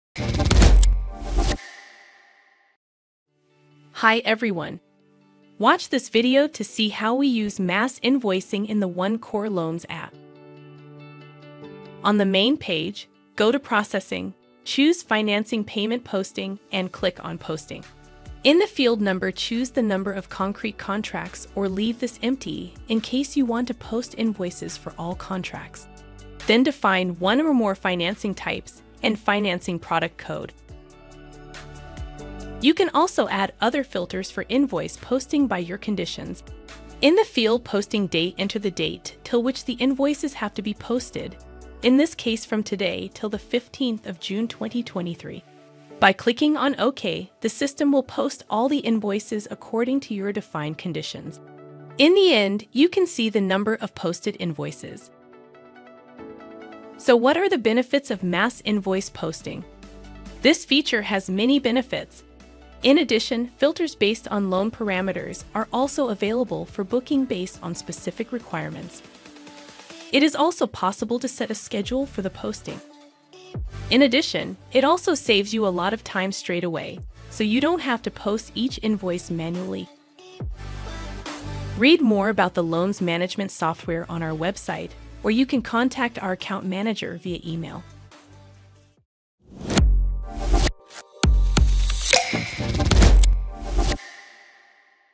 OneCore Loans App – Mass invoices posting | OneCore Apps
So, what are the benefits of mass invoice posting?

It is also possible to set a schedule for the posting.

In addition, it also saves you a lot of time straight away, so you don't have to post each invoice manually.